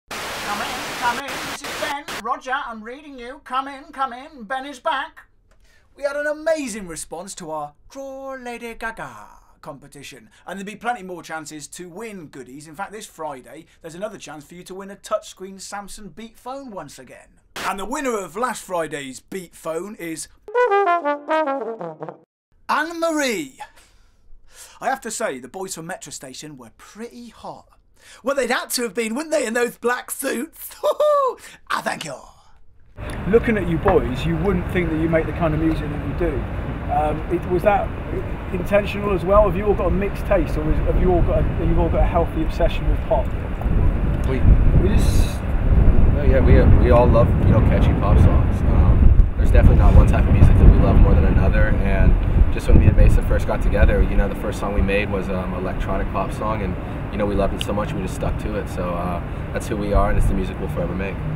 Metro Station, and the latest phone winner
More chit chat from Metro Station, and we reveal the winner of our latest phone competition.